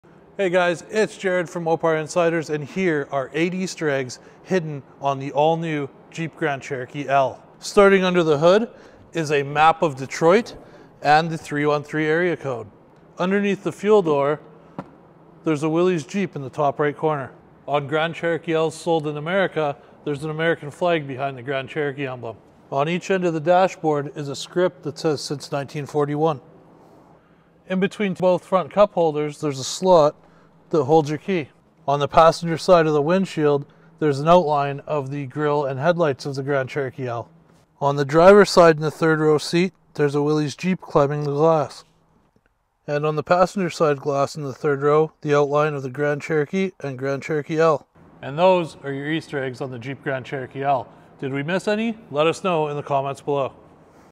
8 Easter Eggs On The Jeep Grand Cherokee L
Here are eight easter eggs on the new Jeep Grand Cherokee L. Some are pretty obvious but some are pretty well hidden. Did we miss any?